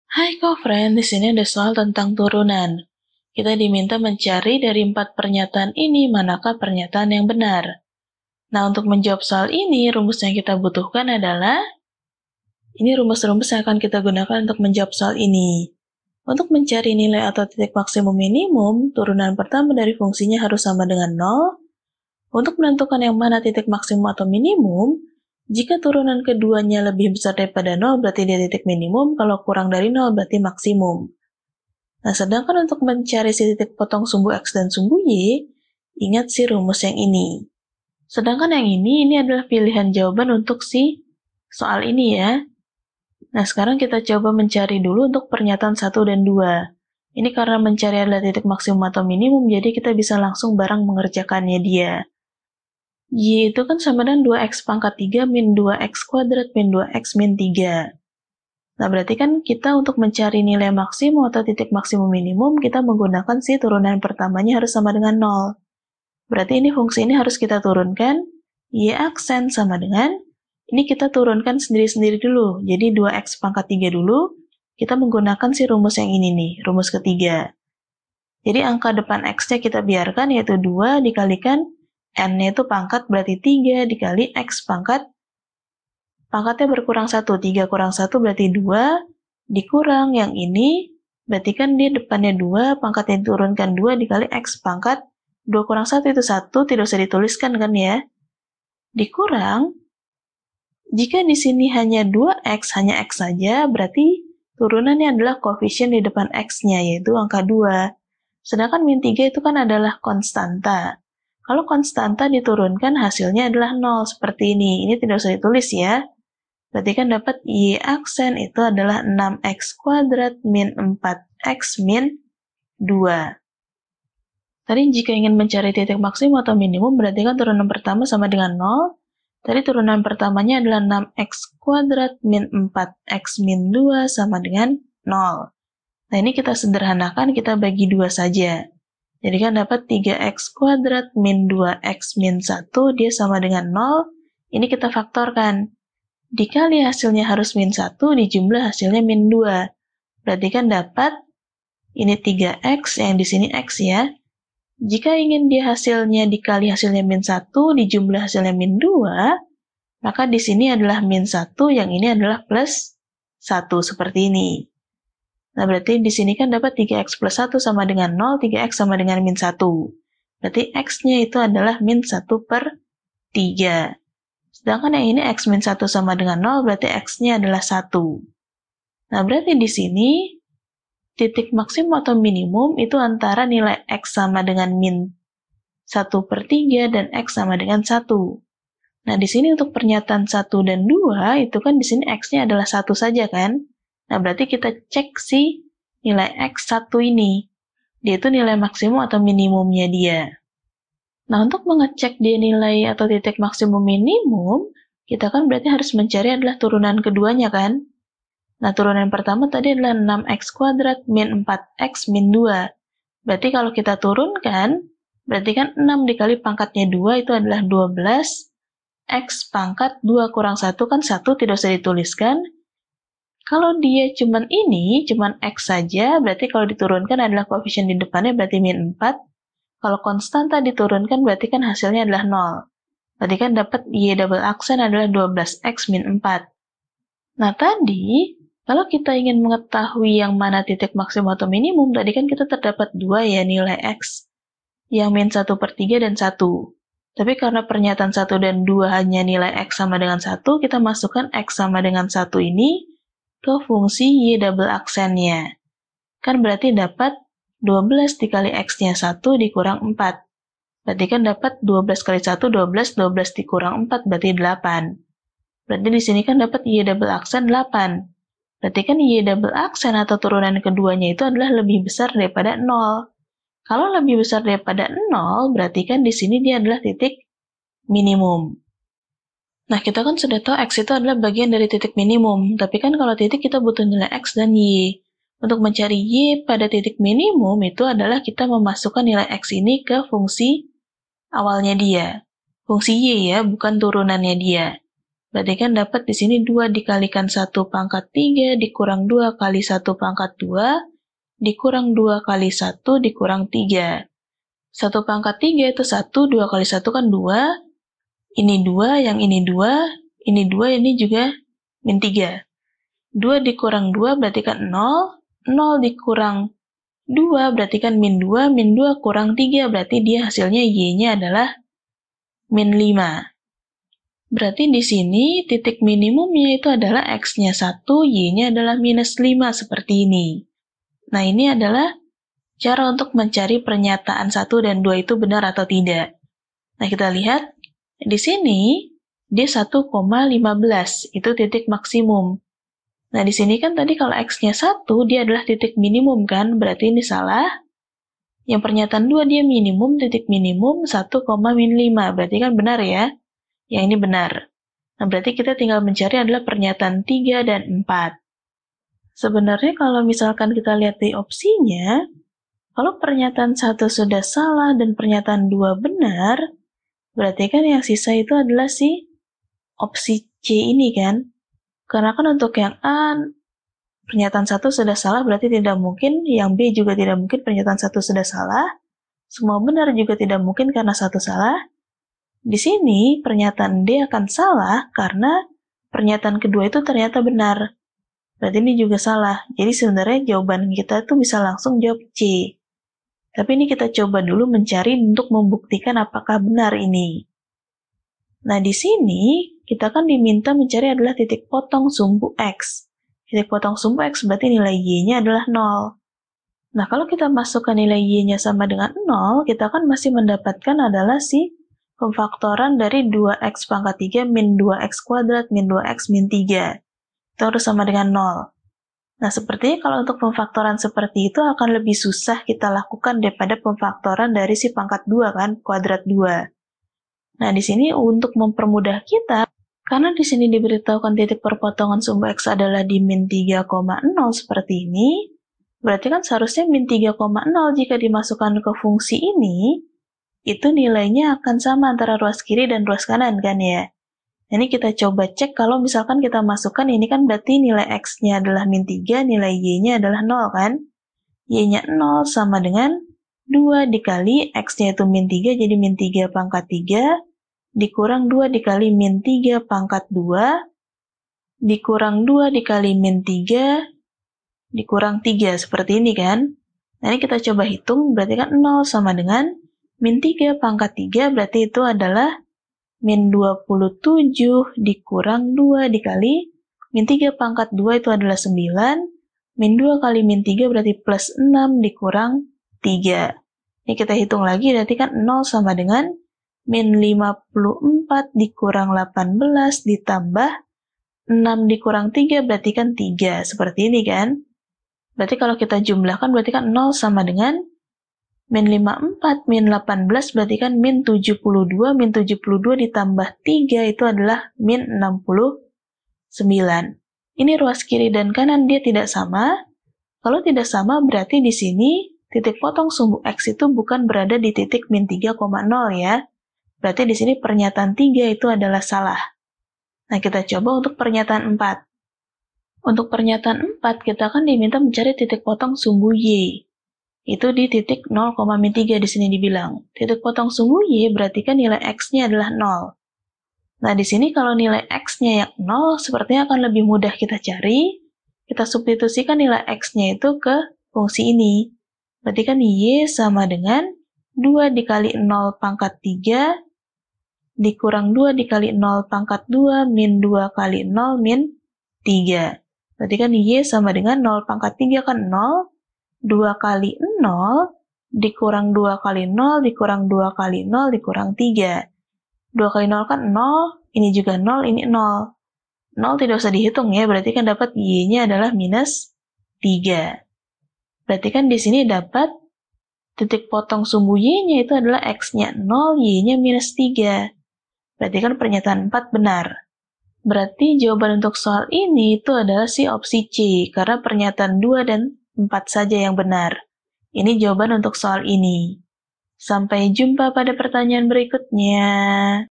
Gunakan Petunjuk C. Jika y=2 x^3-2 x^3-2 x-3 , maka titik .... (1) maksimum (1,15) (2) minimum (1...
Gunakan Petunjuk C. Jika y=2 x^3-2 x^3-2 x-3 , maka titik (1) maksimum (1,15) (2) minimum (1,-5) (3) potong dengan sumbu X pada (-3,0) (4) potong dengan sumbu Y pada (0,-3)

Topik atau Materi: Nilai Maksimum dan Nilai Minimum Fungsi - Turunan - KALKULUS - Matematika Kelas 11 SMA - K-2006

Sumber Soal: Matematika Dasar , Erlangga

00:00 : Bedah Soal
01:00 : Konsep, rumus dan pengertian Nilai Maksimum dan Nilai Minimum Fungsi
01:18 : Langkah penyelesaian soal
08:52 : Jawaban akhir
09:22 : Penutup